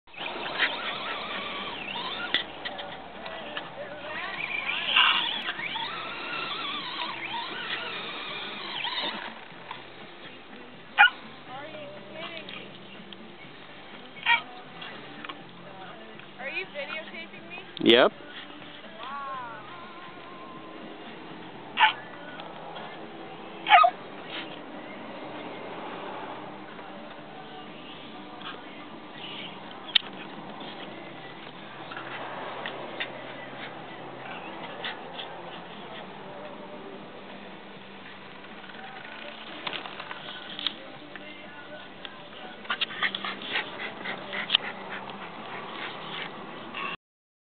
Brushless Traxxas Slash, Sand shut Hillclimb.
My dumb dog wants to chace it, im pretty sure hes constapated